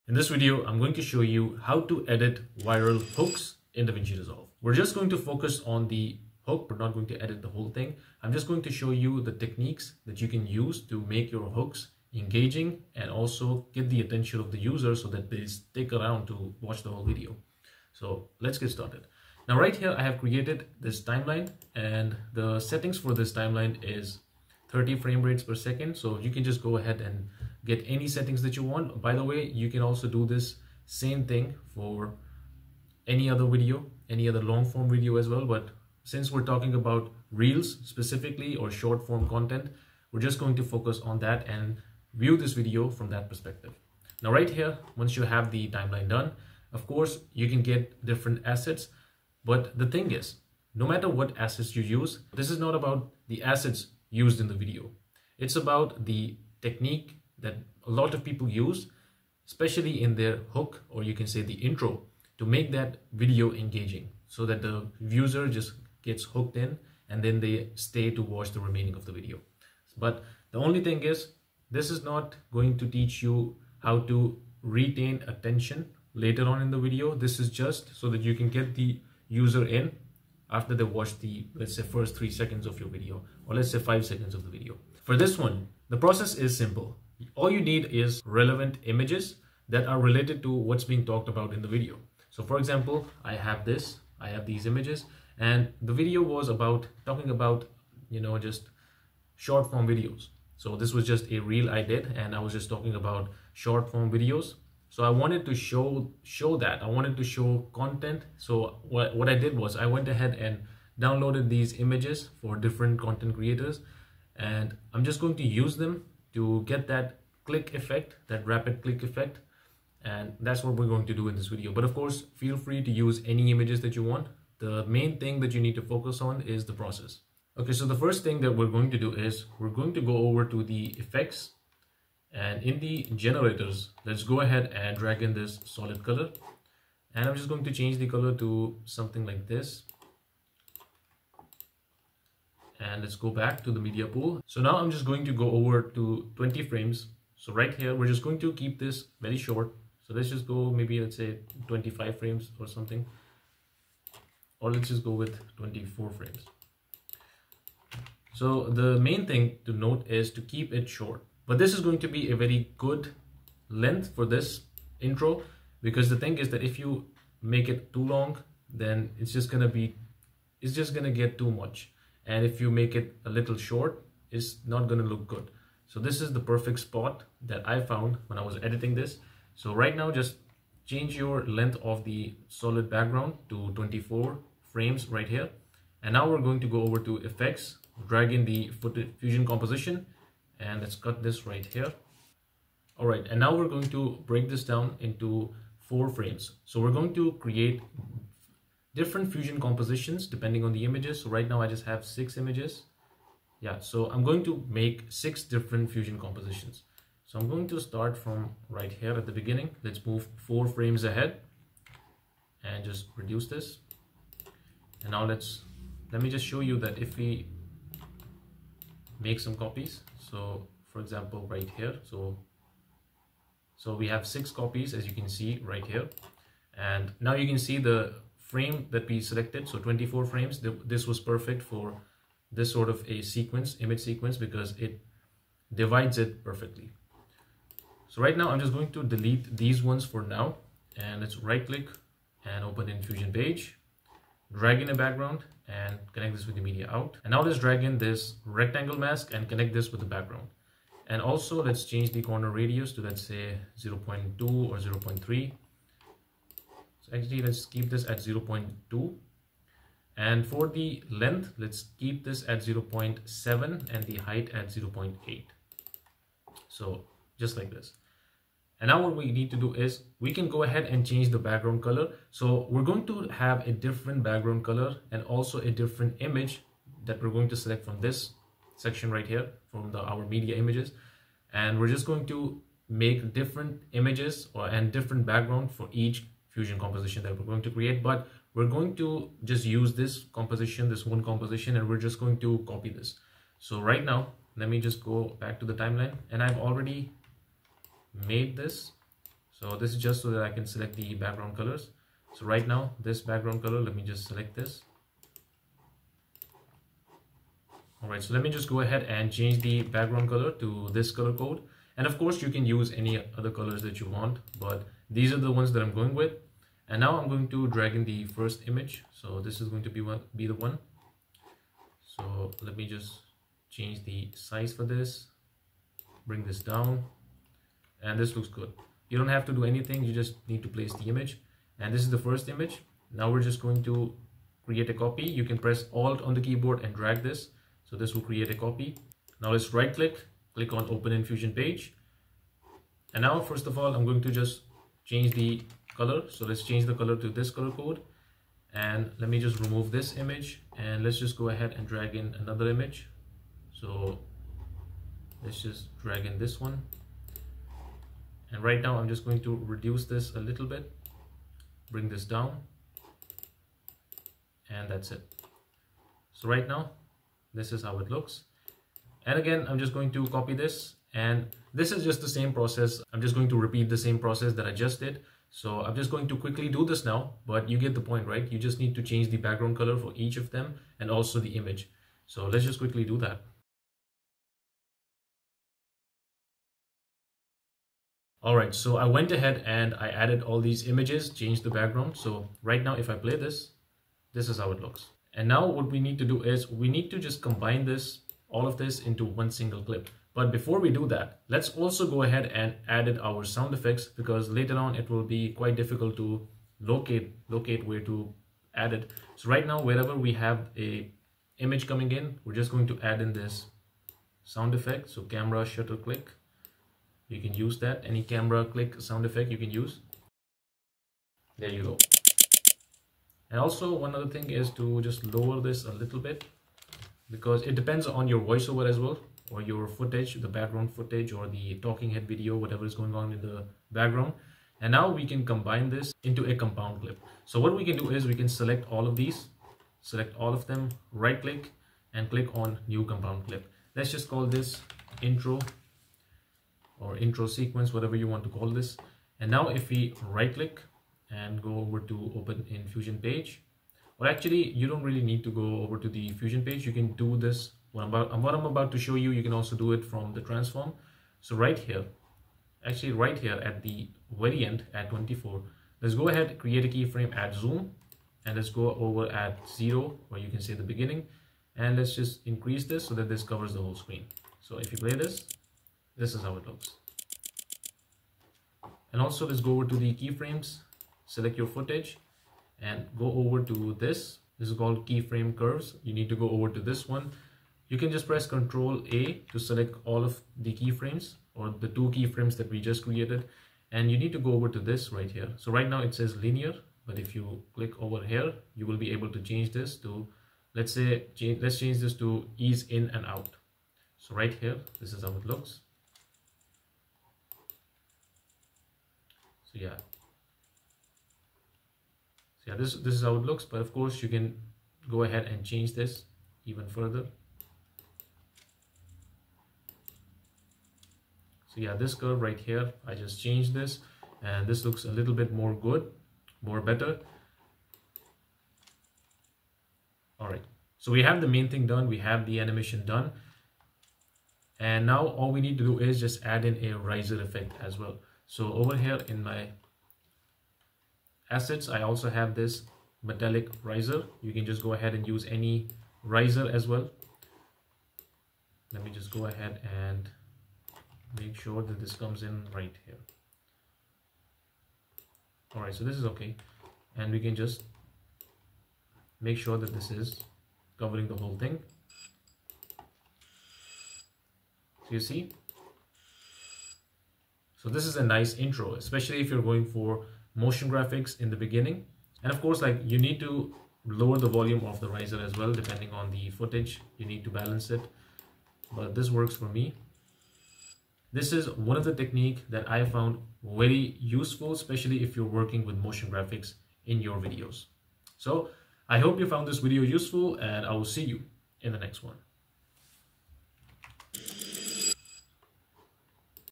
VIRAL REELS HOOK EDITING IN DAVINCI RESOLVE
Most Reels don’t fail because of bad content.
They fail in the first 1–3 seconds.

In this video, I break down how to edit scroll-stopping hooks in DaVinci Resolve—the exact techniques used in viral Reels, Shorts, and TikToks.

📌 Tools used: DaVinci Resolve
📌 Format: Reels / Shorts / TikTok
📌 Skill level: Beginner → Intermediate

Viral editing, retention psychology, and short-form content that actually converts.